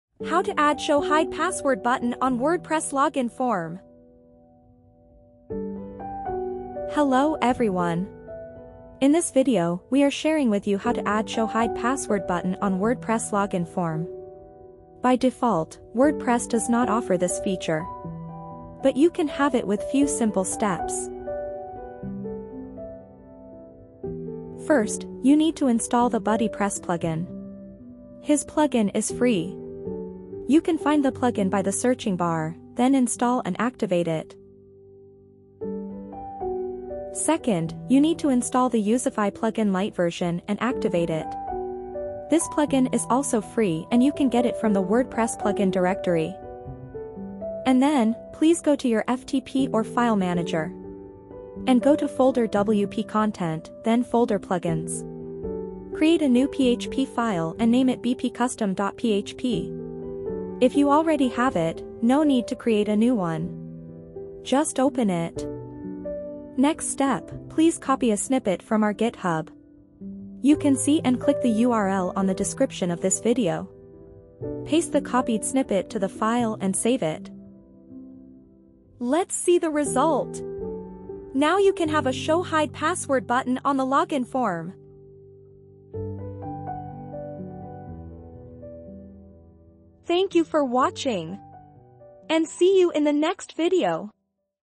How to Add Show Hide Password Button on WordPress Login Form
Hello Everyone. In this video, we are sharing with you How to Add Show Hide Password Button on WordPress Login Form. By default, WordPress does not offer this feature. But you can have it  with few simple steps.

00:00 Introduction
00:24 Install BuddyPress
00:35 Install Youzify Lite Version
00:47 Create bp-custom.php file
01:06 Copy the snippet
01:12 Paste the snippet on to bp-custom.php
01:18 Let's see the Results